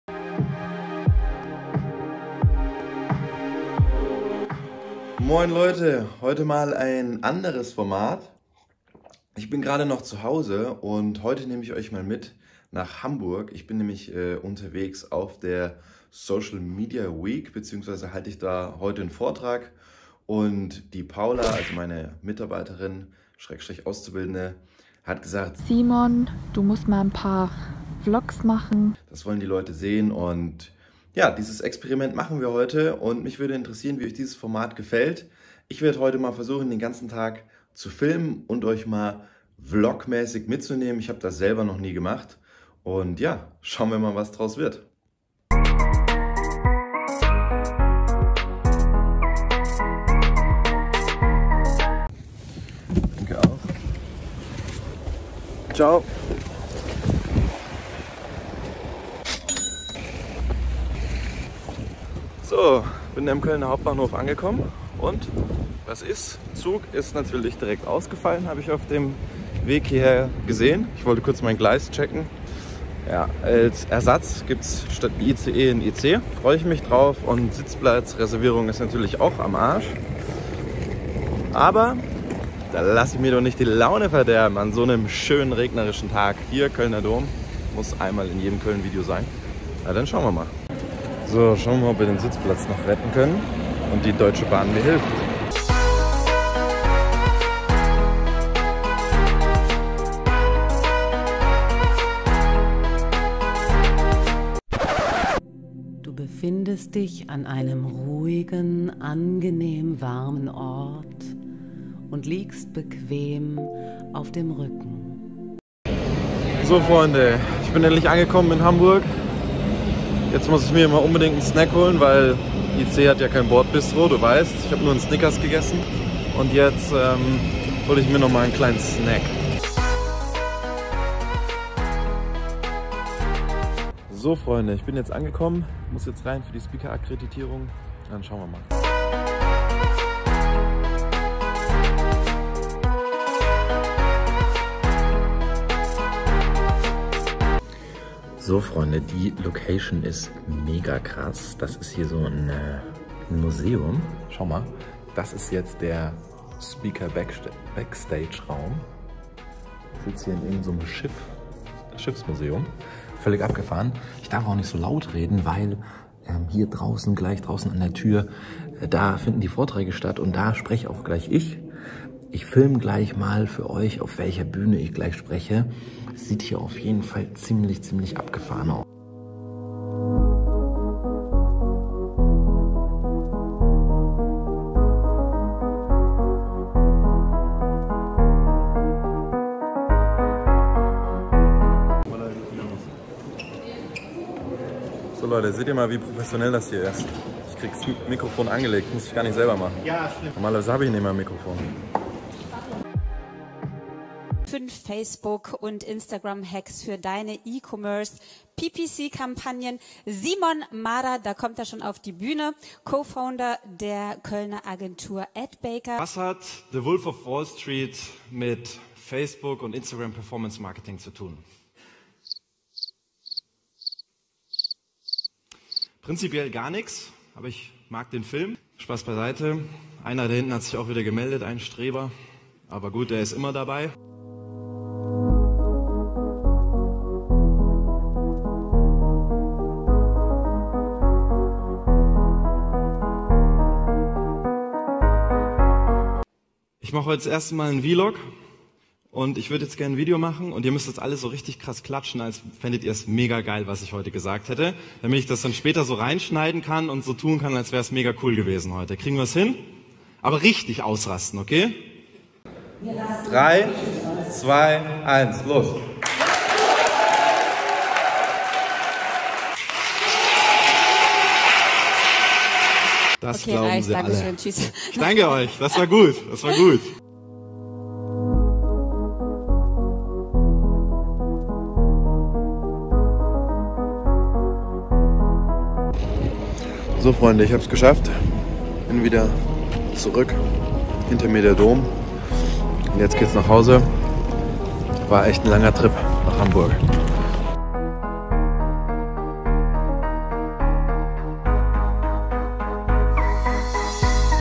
❌ VLOG// Hamburg SMWHH 🚀
▶️Moin Leute,

heute habe ich für Euch meinen aller ersten Vlog! Ich fahre nach Hamburg zu der Social Media Week in Hamburg und nehme Euch mit.

Ich hoffe ihr findet das Format cool! Schreibt mir gerne Feedback in die Kommentare, dann werde ich öfter Vlogs für euch drehen.

Gruß und Kuss

Simon

Simon Mader - Co Founder von ADBAKER GmbH